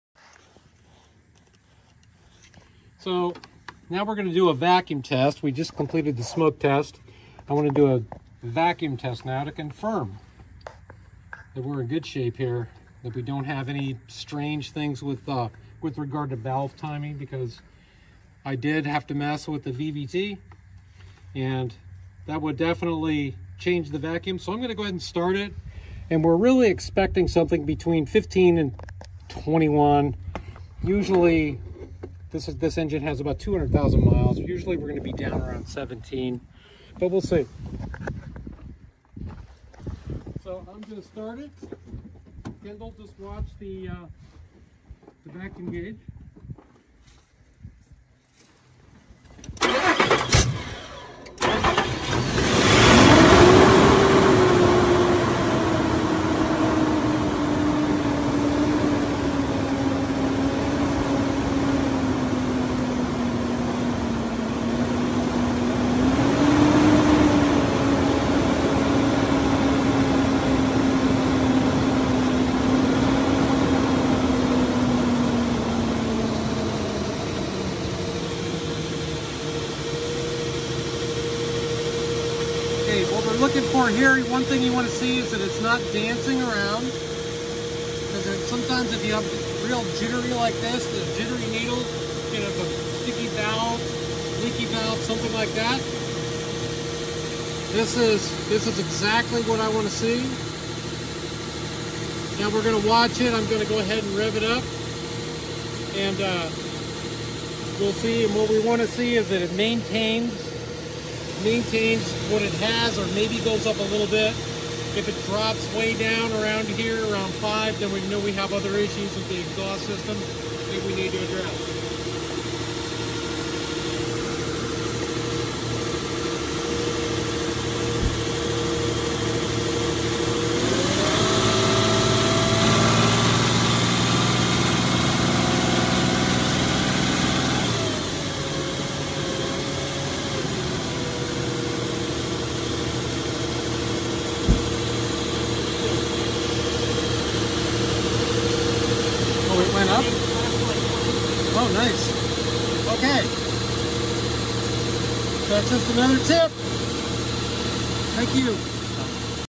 One Test Shows MOST Everything Wrong With Your Engine
Video By:  Grit and Gears DIY

OTC Vacuum Gauge:
OTC Compression Tester
OTC Leakdown Tester:
Autool Smoke Tester:

Following a smoke test, we perform a detailed "vacuum leak test" to confirm the engine's health. This essential "engine diagnosis" demonstrates "how to use a vacuum gauge" effectively, especially after adjusting the "vvt engine" system. Staying on top of "automotive" maintenance helps keep your ride running smoothly. 🛠️